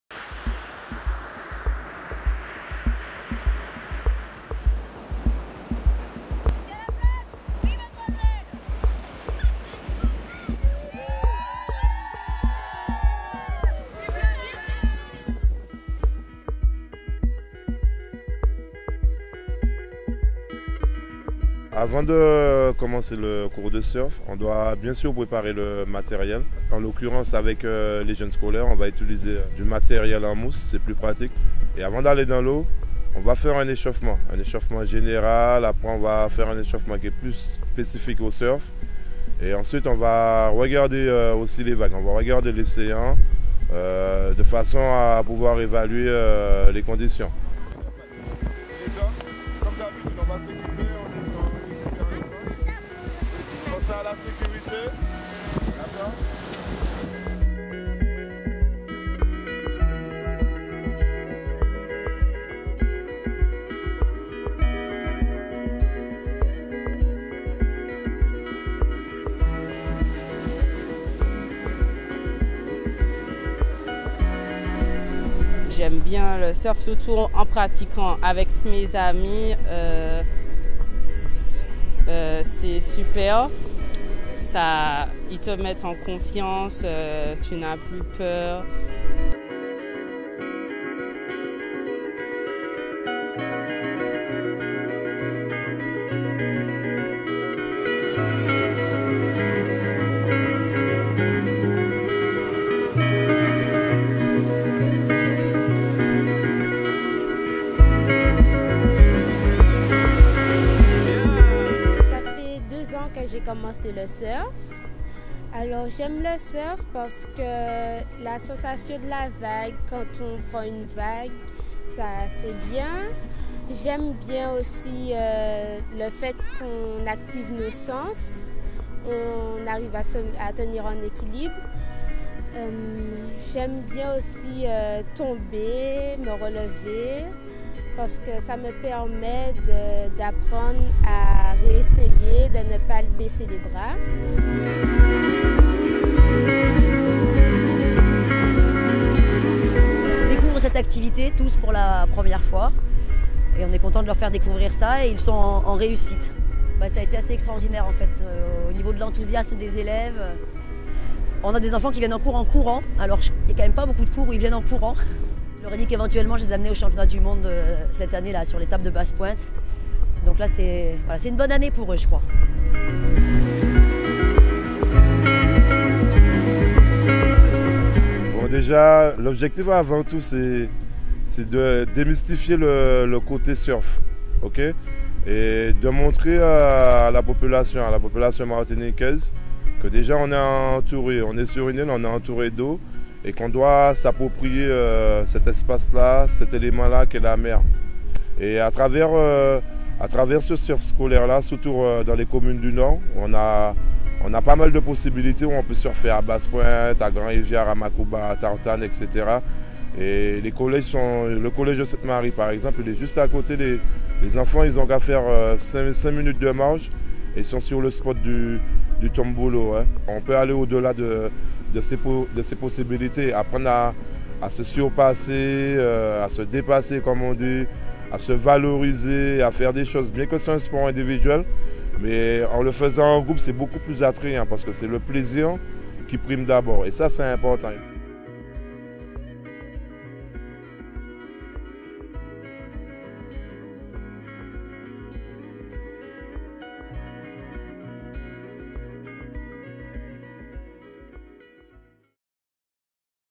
Surf scolaire avec Caraïbe Surf Project
Le Martinique Surf Pro c'est aussi un projet de développement annuel du surf à travers les associations Caraïbe Surf Project et Martinique Surfing qui diffusent cette pratique au plus grand nombre. Quelques images de ces actions de développement avec le surf scolaire. Une activité dont bénéficient plusieurs collèges du Nord Atlantique !